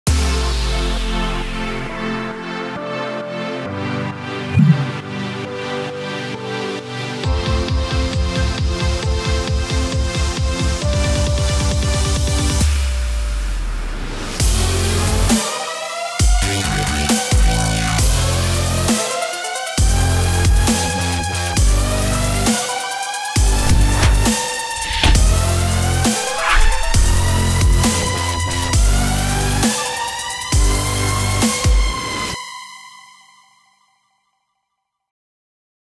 Animated Flex Overlay Package [Twitch/Youtube/Facebook/Co][OBS/SLOBS/XSPLIT]
Welcome to our fully and stylish animated twitch overlay package. Awesome animated game overlay and irl overlay, twitch panels, twitch profile banner and so on. With this package you are perfectly equipped for your stream.
You can use it on any streaming platform such as Twitch, Youtube or Facebook.